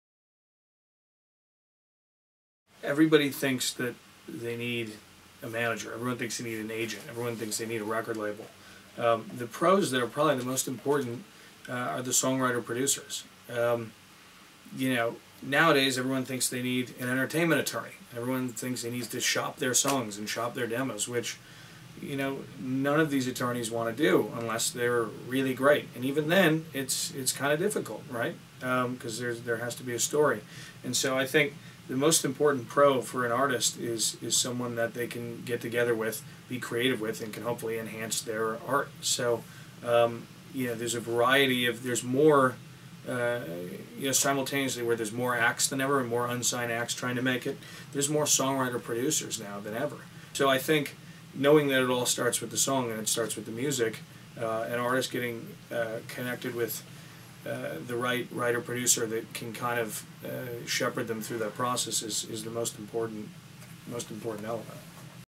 Best Music Pros to Work With
Lucas Keller talks about the importance of working with Songwriter-Producers.